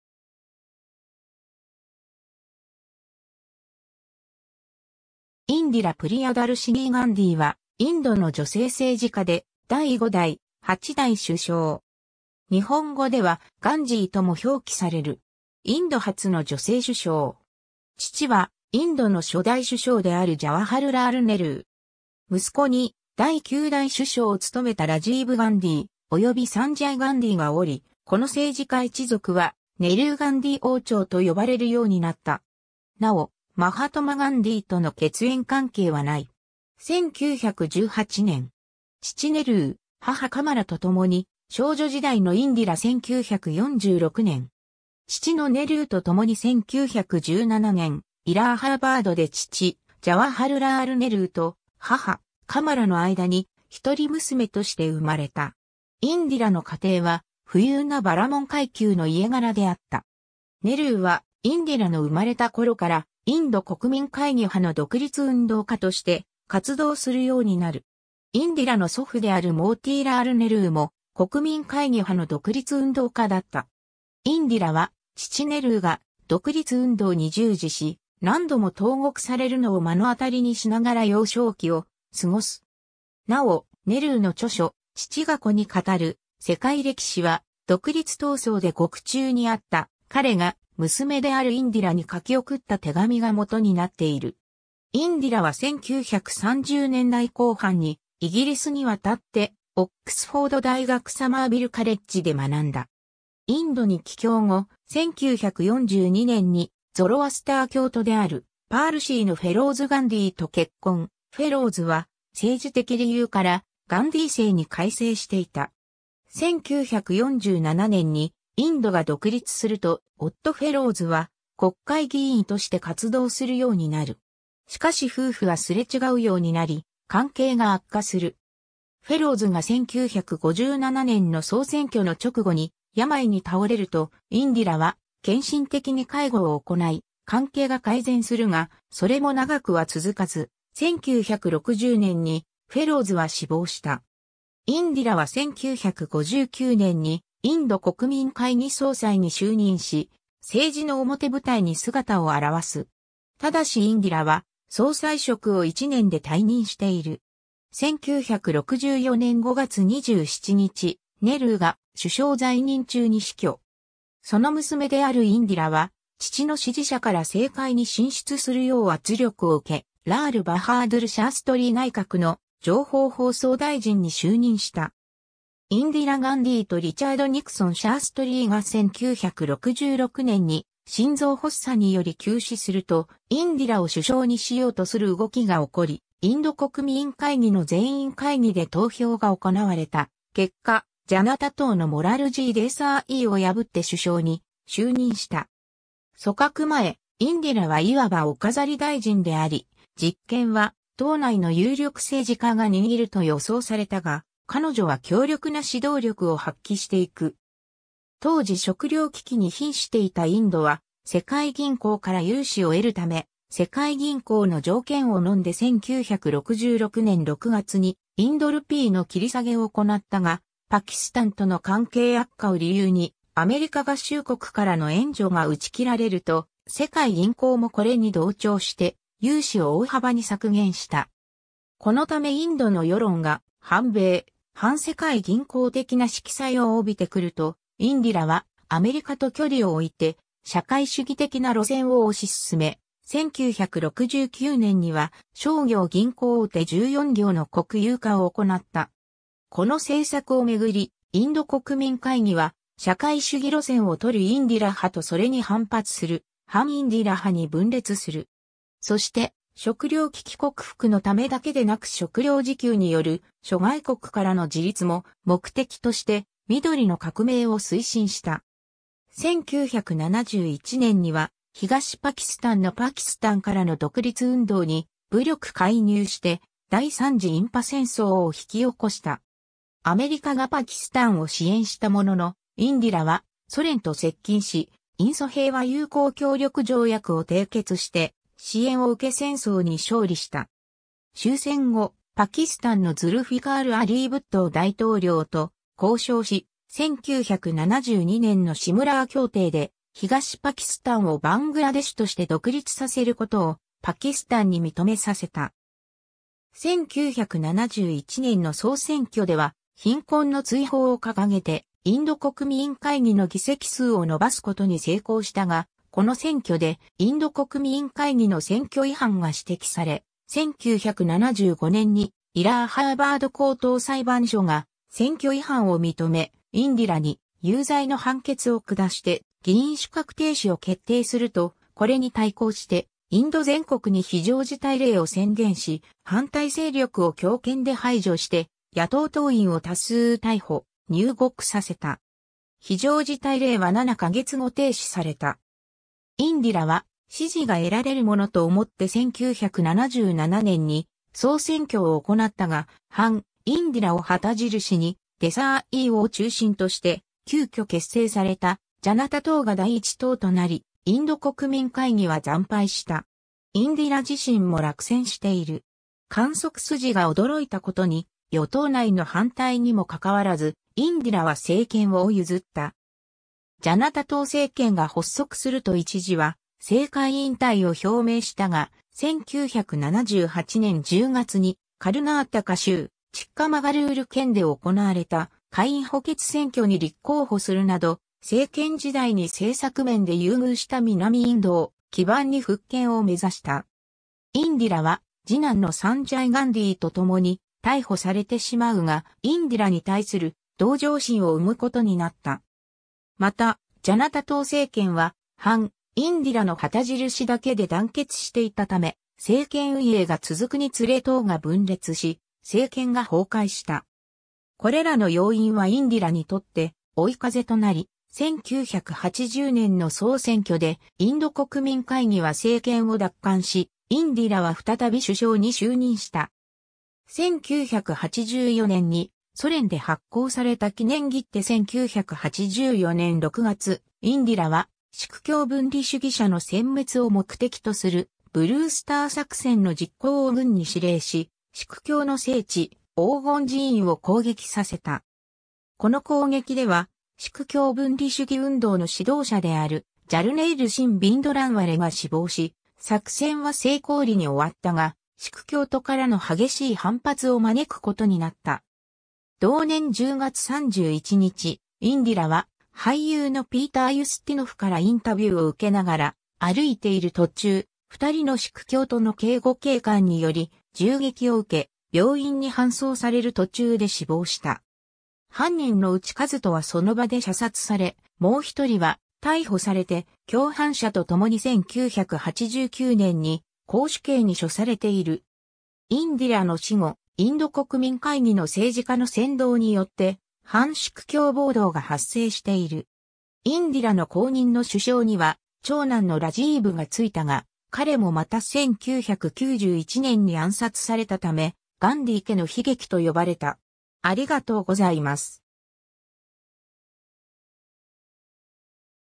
インディラ・ガンディー
ラージヤ・サバー議員
ローク・サバー議員
インディラ・プリヤダルシニー・ガンディー（Indira Priyadarshini Gandhi、इंदिरा प्रियदर्शिनी गाँधी、1917年11月19日 - 1984年10月31日）は、インドの女性政治家で第5代、8代首相。
日本語ではガンジーとも表記される。
 インド初の女性首相。
父はインドの初代首相であるジャワハルラール・ネルー。
息子に第9代首相を務めたラジーヴ・ガンディー、及びサンジャイ・ガンディーがおり、この政治家一族は「ネルー・ガンディー王朝」と呼ばれるようになった。
なお、マハトマ・ガンディーとの血縁関係はない。
 1918年。
父ネルー、母カマラとともに 少女時代のインディラ 1946年。
父のネルーとともに 1917年、イラーハーバードで父ジャワハルラール・ネルーと母カマラの間に一人娘として生まれた。
インディラの家庭は富裕なバラモン階級の家柄であった。
ネルーはインディラの生まれたころからインド国民会議派の独立運動家として活動するようになる。
インディラの祖父（ネルーの父）であるモーティーラール・ネルーも国民会議派の独立運動家だった。
 インディラは父ネルーが独立運動に従事し、何度も投獄されるのを目の当たりにしながら幼少期を過ごす。
なお、ネルーの著書『父が子に語る世界歴史』は、独立闘争で獄中にあった彼が、娘であるインディラに書き送った手紙が基になっている。
 インディラは1930年代後半にイギリスに渡ってオックスフォード大学サマーヴィル・カレッジで学んだ。
インドに帰郷後、1942年にゾロアスター教徒である パールシー(ペルシャ系インド人)のフェローズ・ガンディーと結婚、フェローズは政治的理由からガンディー姓に改姓していた。
 1947年にインドが独立すると、夫フェローズは国会議員として活動するようになる。
しかし夫婦はすれ違うようになり関係が悪化する。
フェローズが1957年の総選挙の直後に病に倒れると、インディラは献身的に介護を行い、関係が改善するが、それも長くは続かず、1960年にフェローズは死亡した。
 インディラは1959年にインド国民会議総裁に就任し、政治の表舞台に姿を現す。
ただしインディラは総裁職を1年で退任している。
 1964年5月27日、ネルーが首相在任中に死去。
その娘であるインディラは父の支持者から政界に進出するよう圧力を受け、ラール・バハードゥル・シャーストリー内閣の情報放送大臣に就任した。
 インディラ・ガンディーとリチャード・ニクソン シャーストリーが1966年に心臓発作により急死すると、インディラを首相にしようとする動きが起こり、インド国民会議の全員会議で投票が行われた結果、ジャナタ党のモラルジー・デーサーイーを破って首相に就任した。
組閣前、インディラはいわばお飾り大臣であり、実権は党内の有力政治家が握ると予想されたが、彼女は強力な指導力を発揮していく。
 当時食糧危機に瀕していたインドは世界銀行から融資を得るため、世界銀行の条件を呑んで1966年6月にインド・ルピーの切り下げを行ったが、パキスタンとの関係悪化を理由にアメリカ合衆国からの援助が打ち切られると、世界銀行もこれに同調して融資を大幅に削減した。
このためインドの世論が反米・反世界銀行的な色彩を帯びてくると、インディラはアメリカと距離を置いて社会主義的な路線を推し進め、1969年には商業銀行大手14行の国有化を行った。
この政策をめぐりインド国民会議は社会主義路線を採るインディラ派とそれに反発する反インディラ派に分裂する。
そして食糧危機克服のためだけでなく食糧自給による諸外国からの自立も目的として緑の革命を推進した。
 1971年には東パキスタンのパキスタンからの独立運動に武力介入して第三次印パ戦争を引き起こした。
アメリカがパキスタンを支援したものの、インディラはソ連と接近し、印ソ平和友好協力条約を締結して支援を受け戦争に勝利した。
終戦後、パキスタンのズルフィカール・アリー・ブットー大統領と交渉し、1972年のシムラー協定で東パキスタンをバングラデシュとして独立させることをパキスタンに認めさせた。
 1971年の総選挙では「貧困の追放」を掲げてインド国民会議の議席数を伸ばすことに成功したが、この選挙でインド国民会...